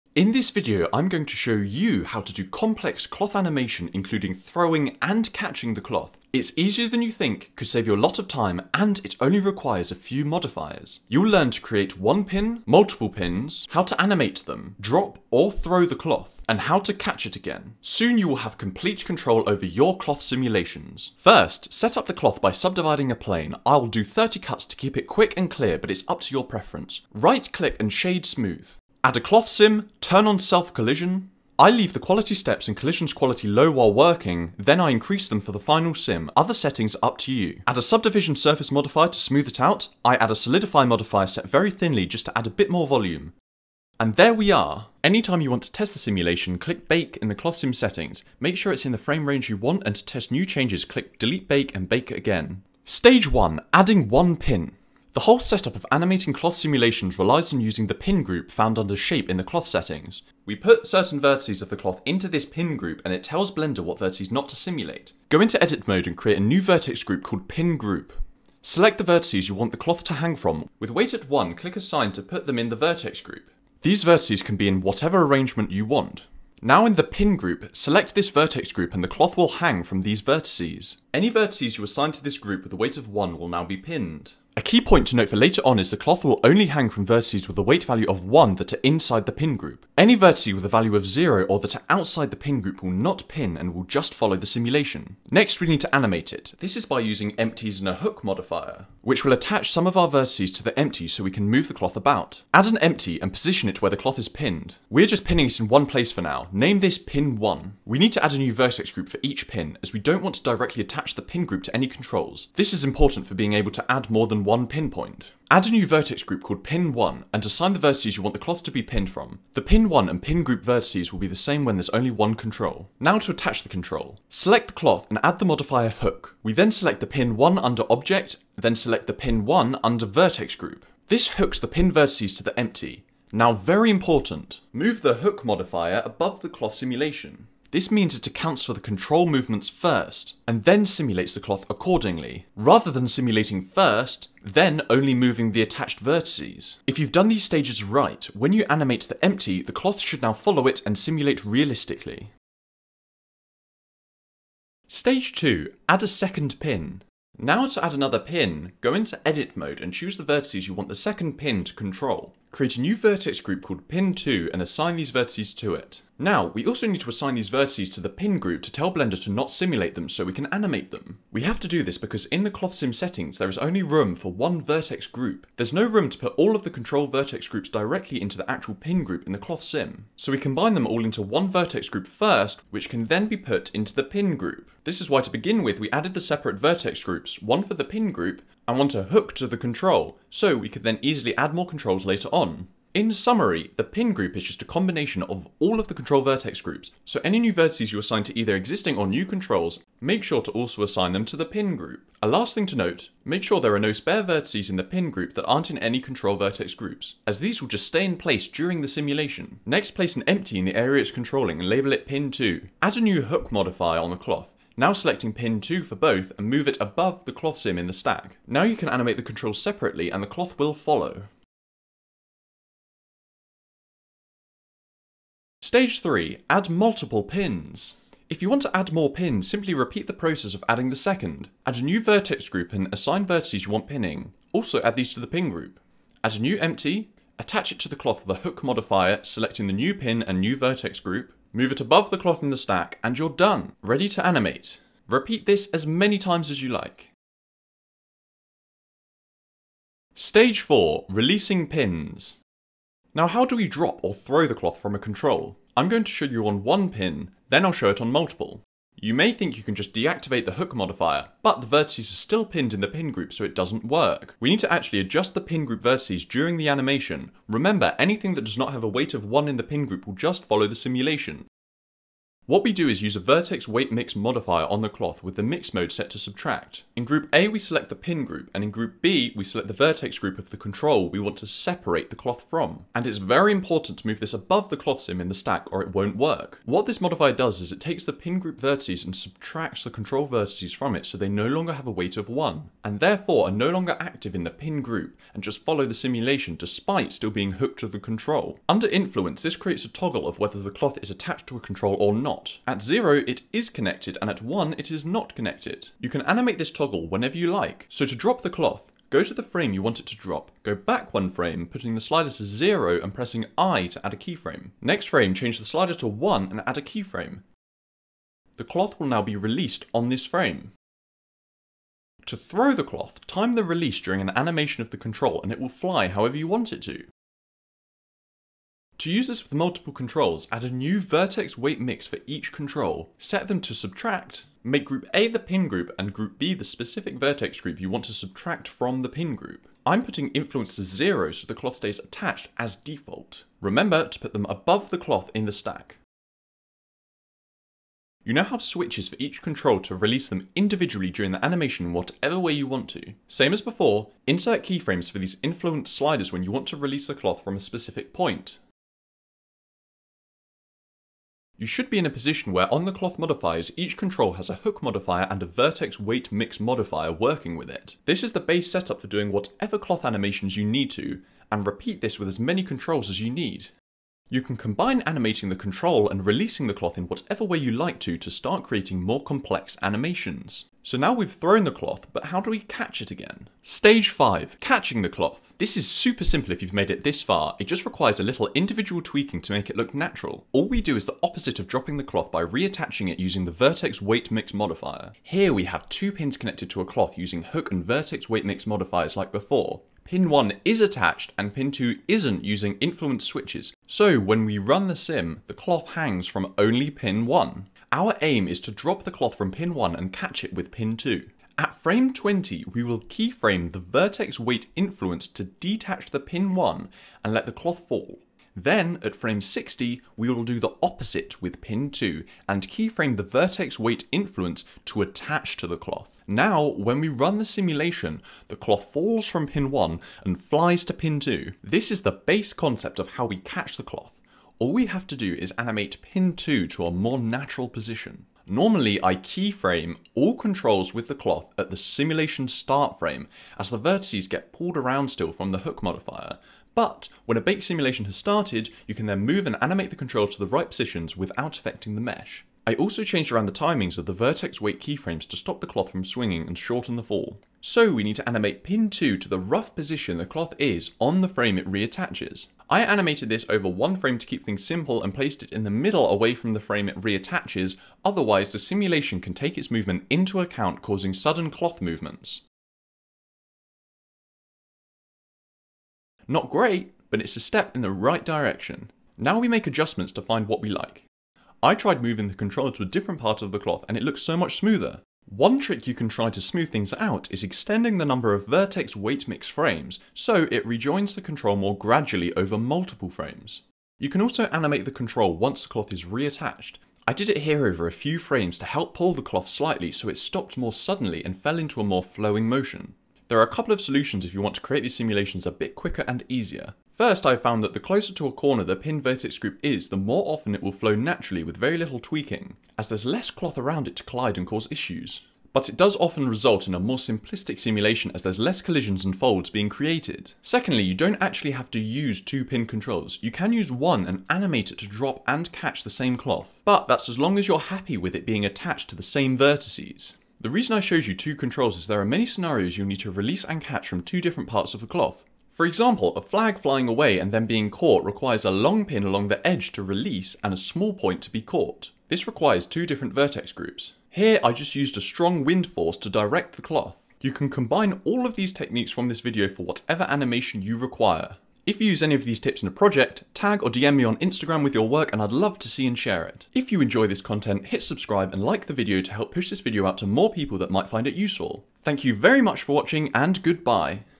Learn Complex CLOTH ANIMATION in under 10 MINUTES! | Blender 3D Tutorial
Improve your Cloth Simulation Animations in this Blender 3D tutorial! Learn complex Blender Cloth Simulation Animation giving you the tools to use your own creativity to make awesome CGI! This is created in Blender free 3D Software so anyone can learn how to!

Feel free to follow my other social media pages too!

Timecodes:
0:00 - Intro
0:21 - Set Up Cloth Simulation
0:52 - Animate Cloth Simulation with ONE PIN
2:35 - Animate Cloth Simulation with TWO PINS
3:45 - Animate Cloth Simulation with MULTIPLE PINS
4:10 - How to DROP or THROW a cloth simulation
6:20 - How to CATCH a cloth simulation

8:22 - CATCH cloth simulation QUICK & EASY TIPS

9:10 - Thank you for Watching!